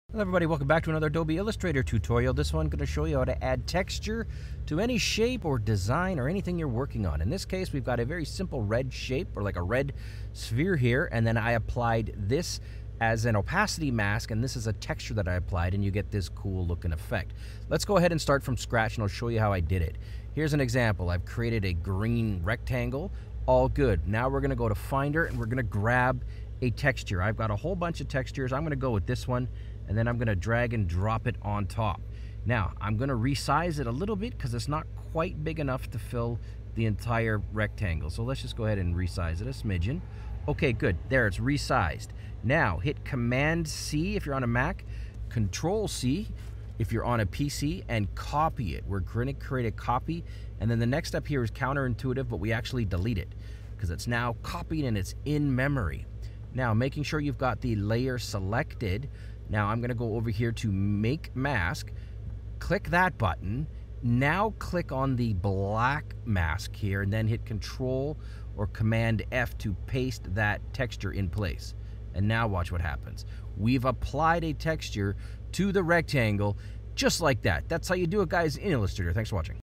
How to Add Texture And Textured Effect to Any Shape Or Design - Adobe Illustrator
In this short tutorial I show you how to add a texture or textured effect to any design using Adobe Illustrator.  We use the transparency panel to create the result.